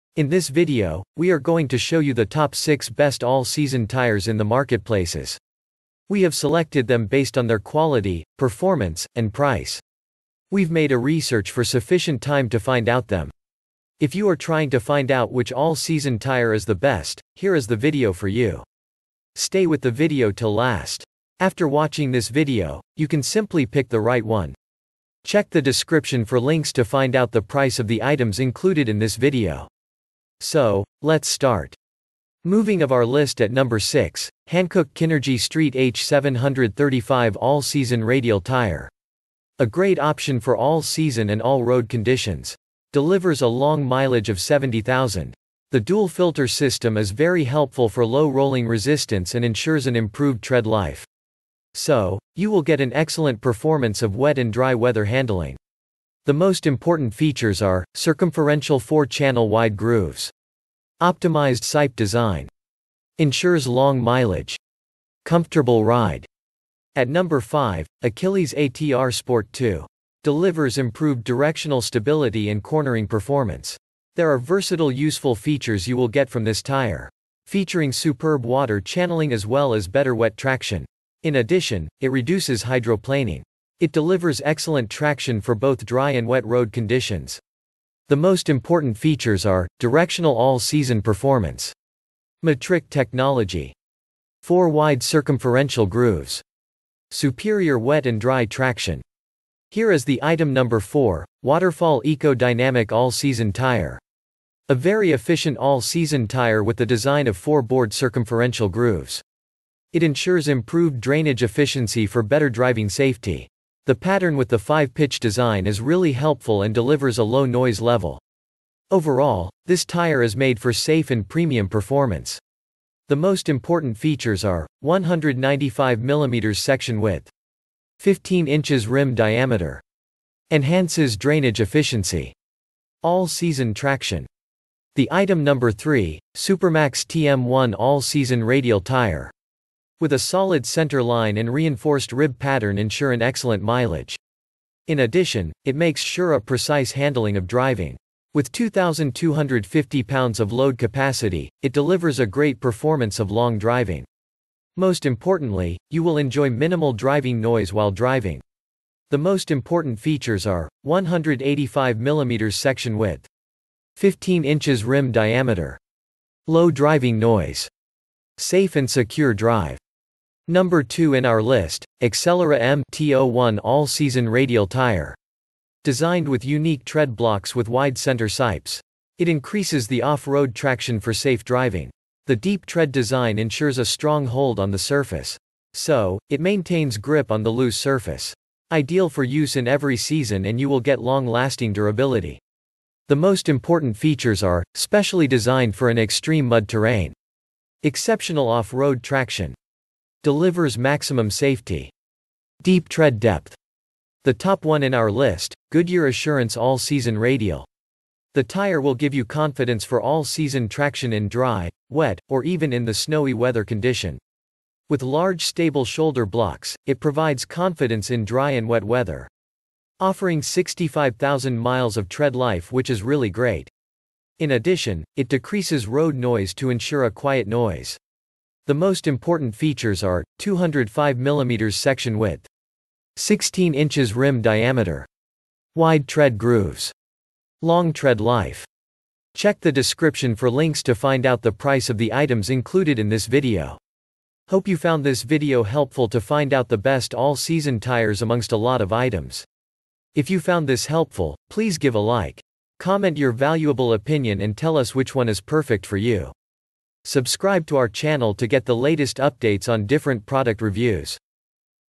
Top 6 Best All Season Tires 2020
6. Hankook Kinergy ST H735 All-Season

5. Achilles ATR Sport 2

4. Waterfall Eco Dynamic All-Season Tire

3. Supermax TM-1 All-Season Radial Tire

2. Accelera M/T-01 All-Season Radial Tire

1. Goodyear Assurance All-Season Radial